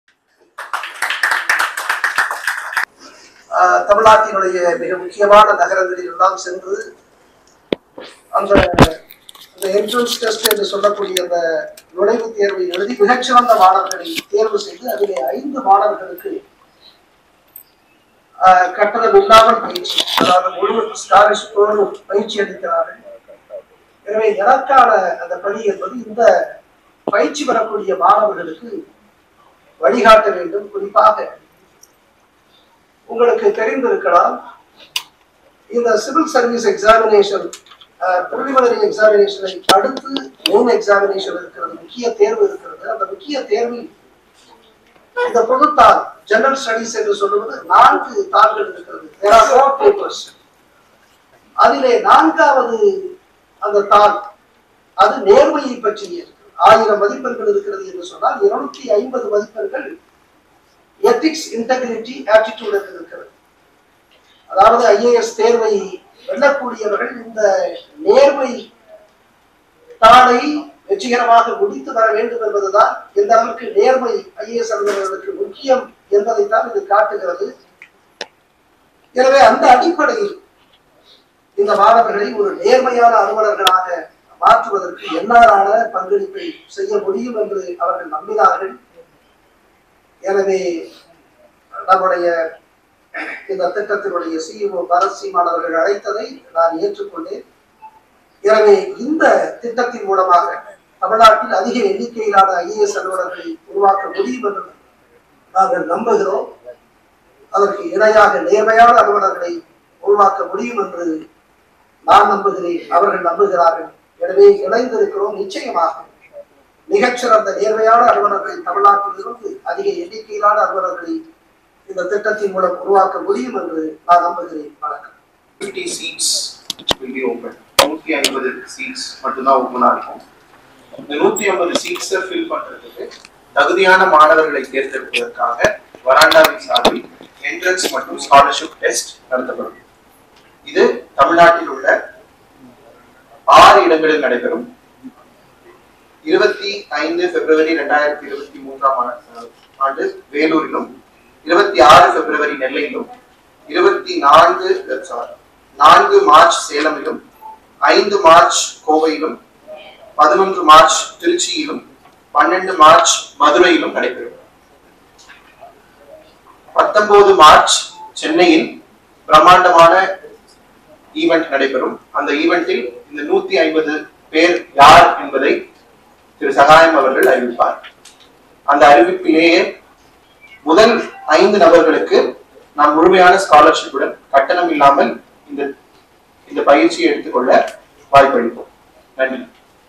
Launch of Academy of Civil Service Aspirants | India’s first residential programme for Civil Service
Veranda IAS Academy launched Academy of Civil Service Aspirants (ACSA)ACSA is India’s first holistic residential programme for Civil Service Aspirants by Veranda IAS